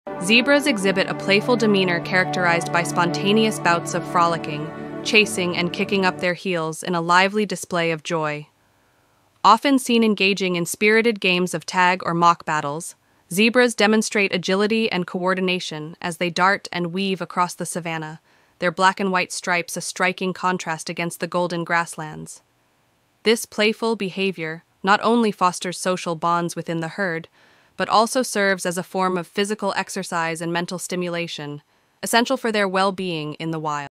Zebras' Playful Style: Glimpse of Jungle Joy
"Step into the playful world of zebras as they romp and frolic in the heart of the savannah! In this YouTube Shorts video, witness the infectious joy and camaraderie among these striped beauties as they engage in playful games and spirited antics. From energetic races to jovial interactions, each moment captures the essence of pure, unbridled fun in the wild. Join us for a front-row seat to the delightful spectacle of zebras at play!"